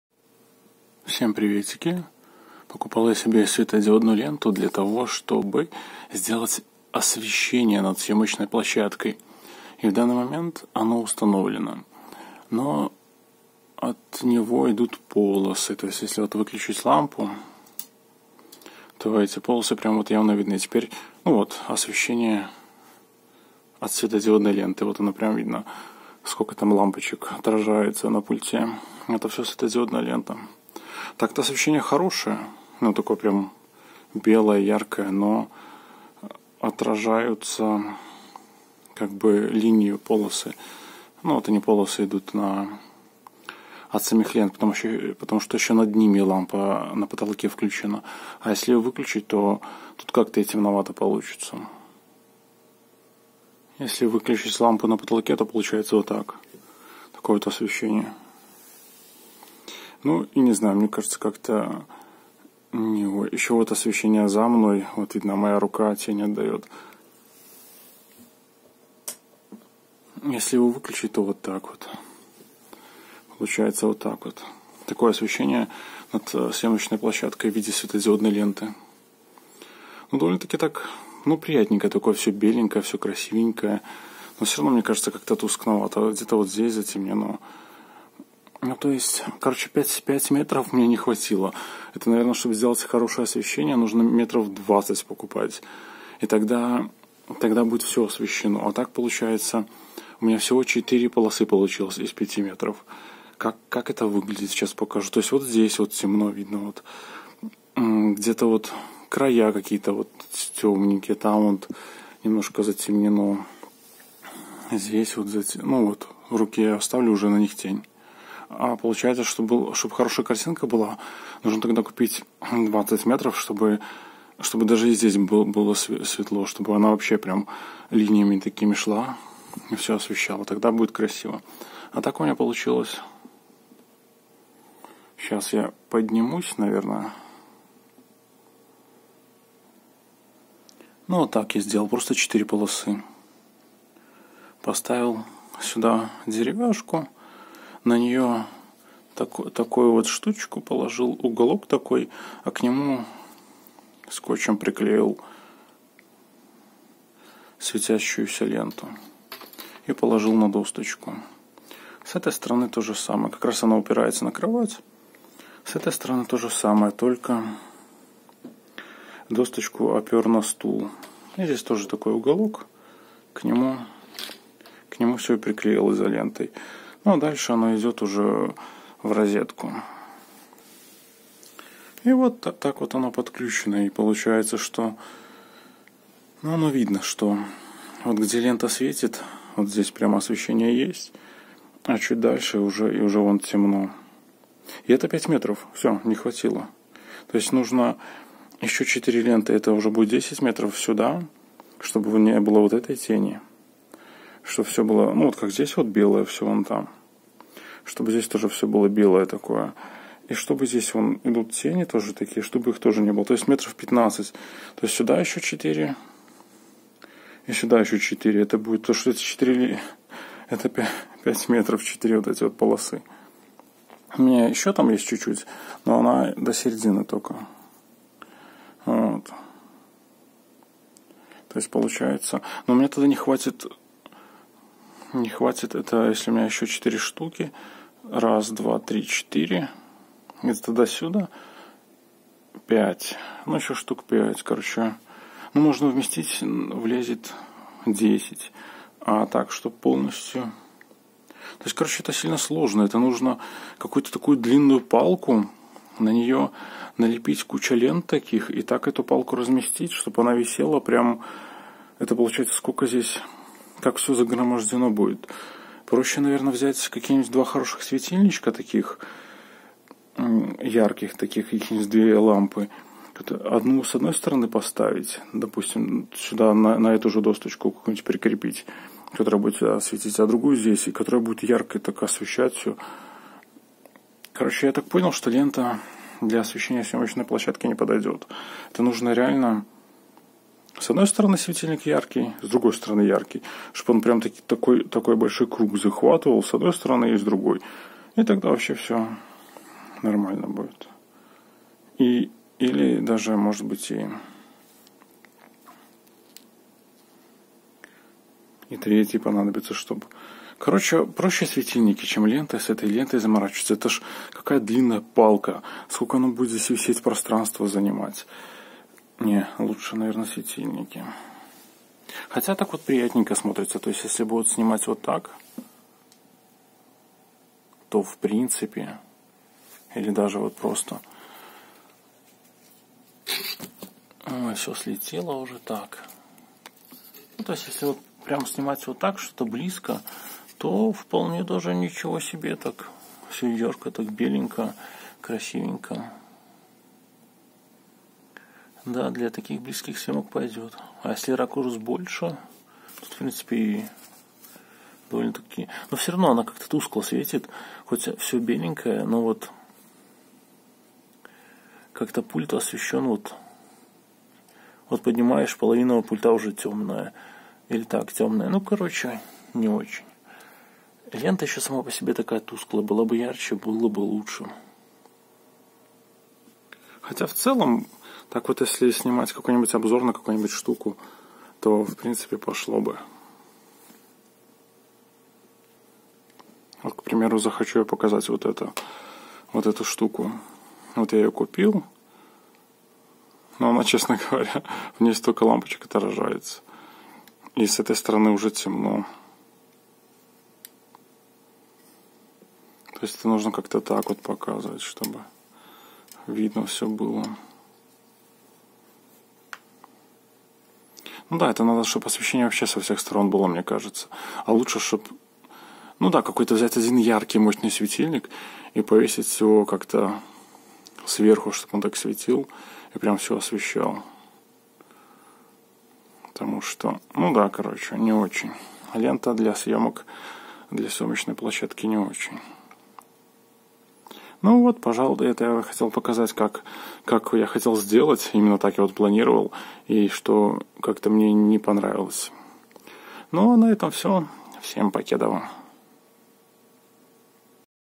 Сделал освещение для съёмочной площадки из светодиодной ленты
Показываю что вышло из светодиодной ленты, которою я использовал в качестве освещения для съёмочной площадки. Я разочаровался в ней. Ещё наверное большую роль сыграло то, что она тусклая сама по себе.